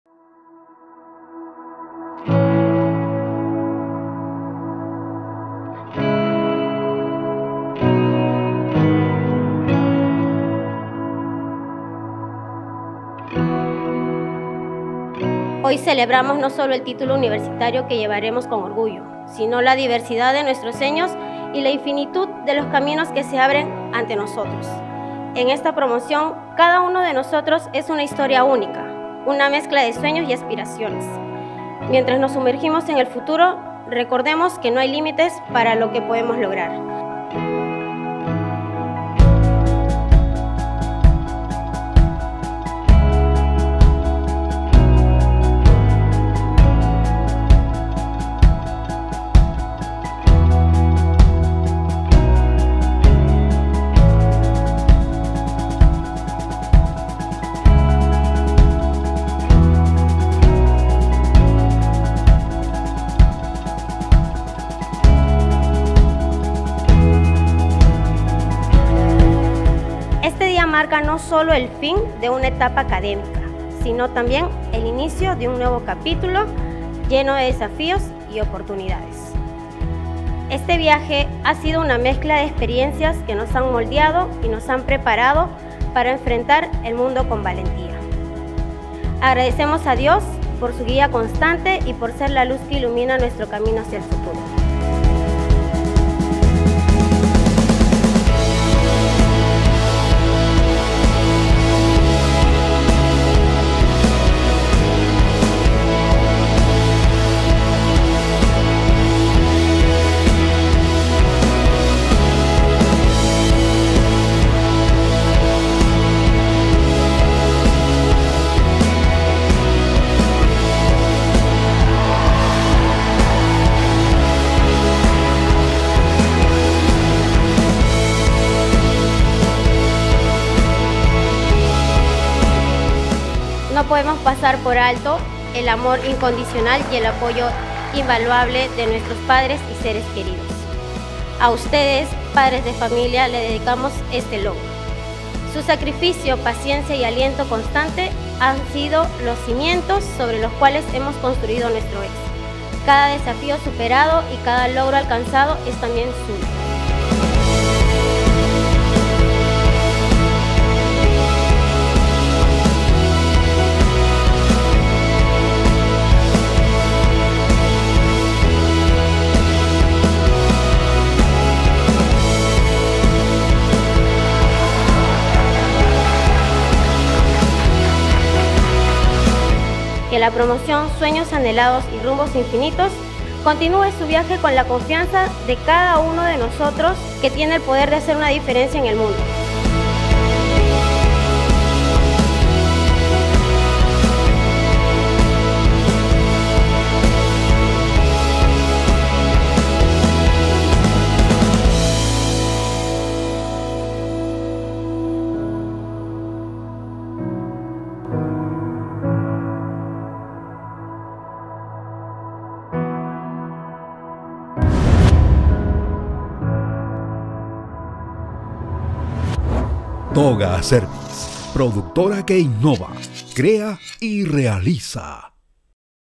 Reel: UTP - Facultad de Administración y Negocios
🎉✨ Revive los destellos de felicidad, éxito y satisfacción de la graduación de los egresados de la UTP, Facultad de Administración y Negocios. 🎓 Inspiración y metas cumplidas en un solo lugar. 🤩🎖
▶ ¿Quieres que tu graduación sea igual de especial? Estamos aquí para hacerlo realidad. Comunícate con nosotros al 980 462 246.